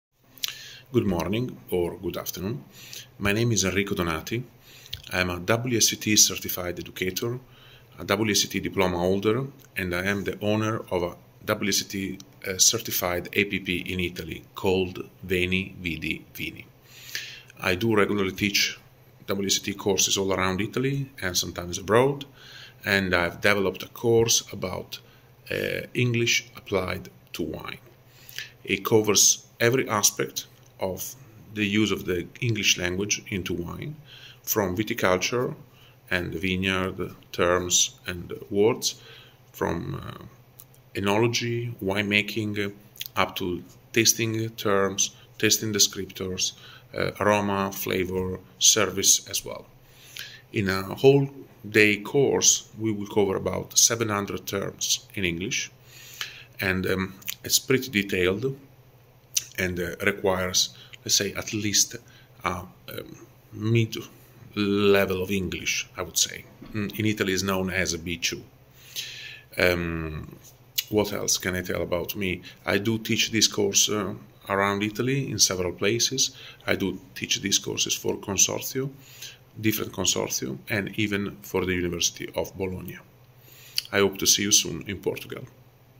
Apresentação Curso Prático Inglês Técnico Aplicado ao Vinho - Enrico Donati
Apresentação do orador Enrico Donati sobre o curso de Inglês Técnico Aplicado ao Vinho